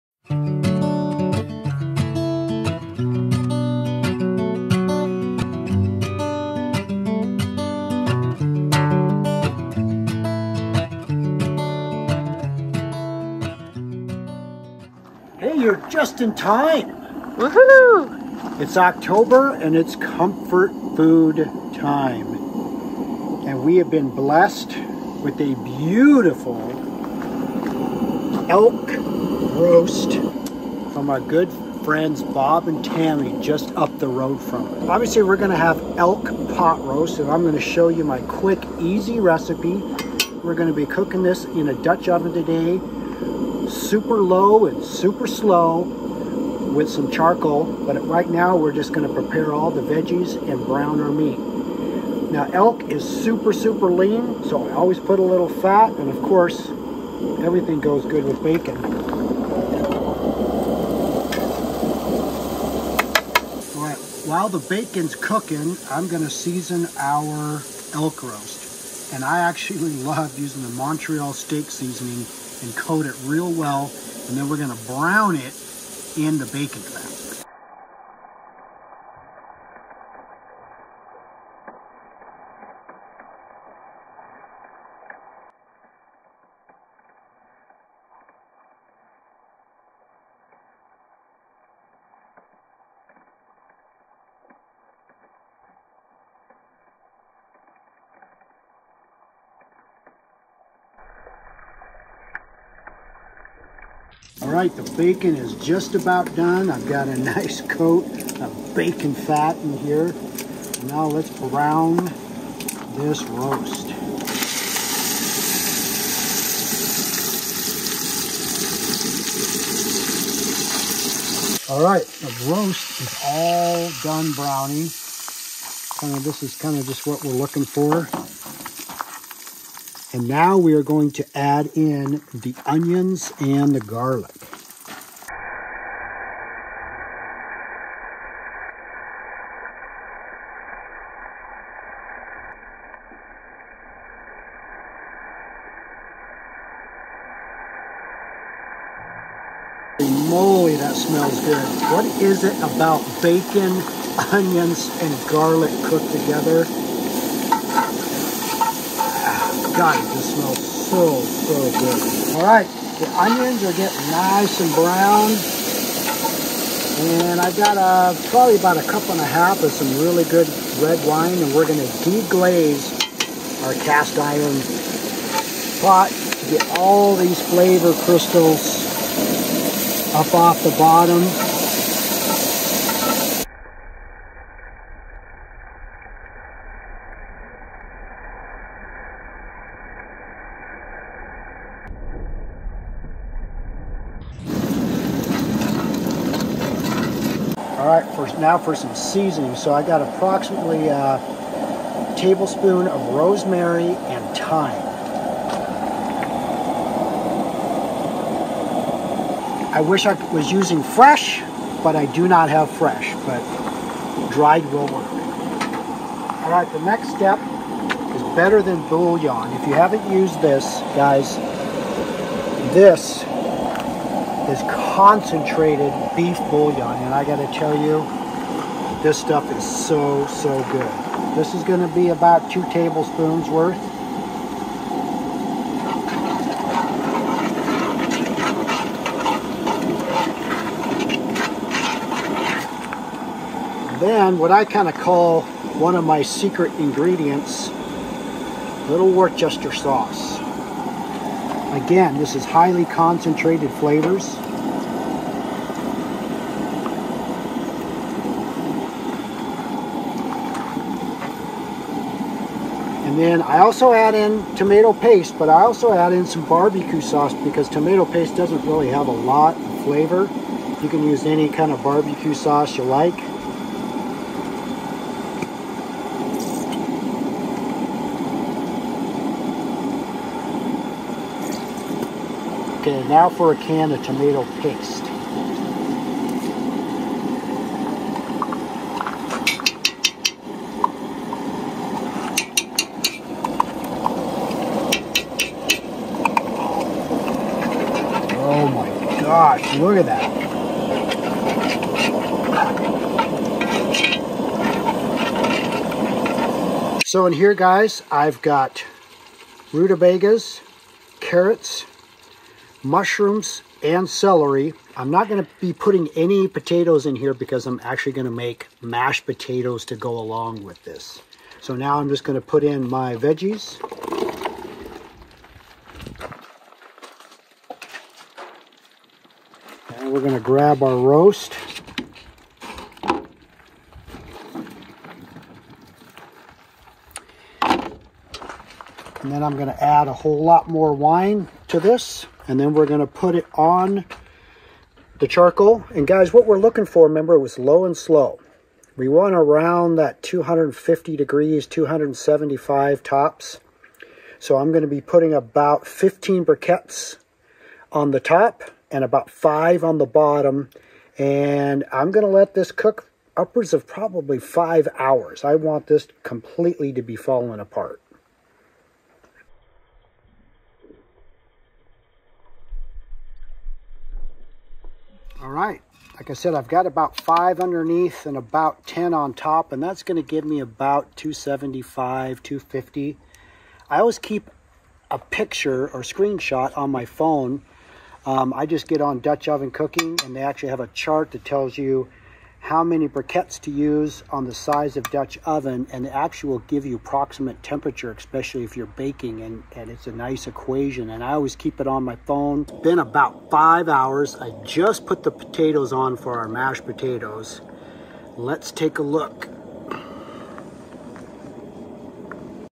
COOKING AN ELK ROAST, IN A DUTCH OVEN, OVER CHARCOAL...WHAT COULD BE BETTER?!
Like pot roast? Don't have an oven??? Rich makes an amazing fall apart Elk roast in the dutch oven over charcoal...AMAZING!